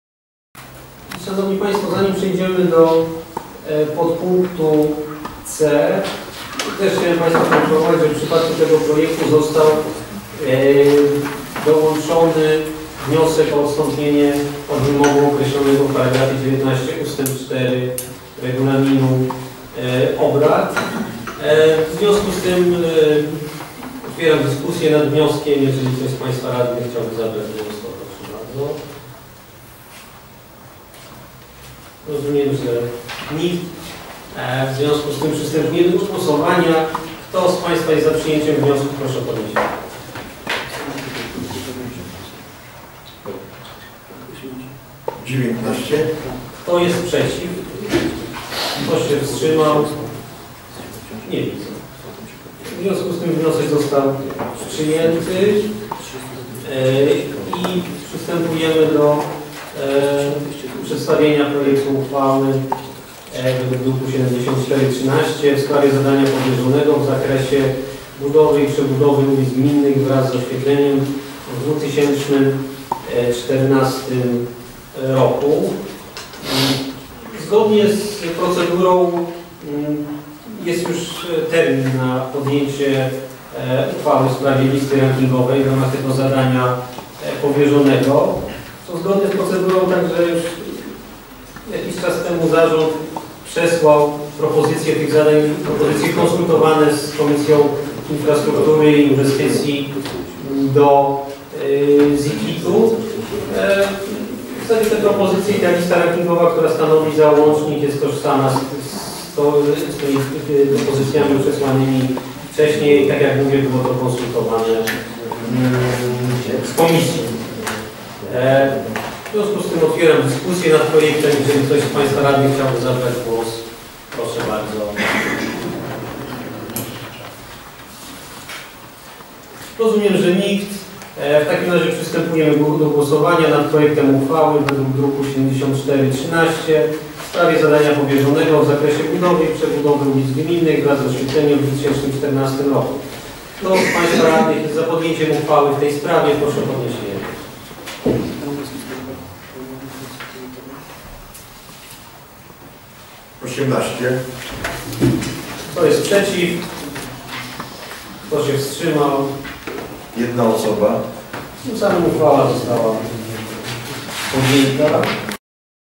RD_XI_26-08-2013_5 budowa i przebudowa ulic gminnych wraz z oświetleniem w 2014
Obrady XXXVI sesji Rady Dzielnicy XI Podgórze Duchackie z dn. 26 sierpnia 2013.

Zadanie powierzone w zakresie "budowy i przebudowy ulic gminnych wraz z oświetleniem" w 2014 roku.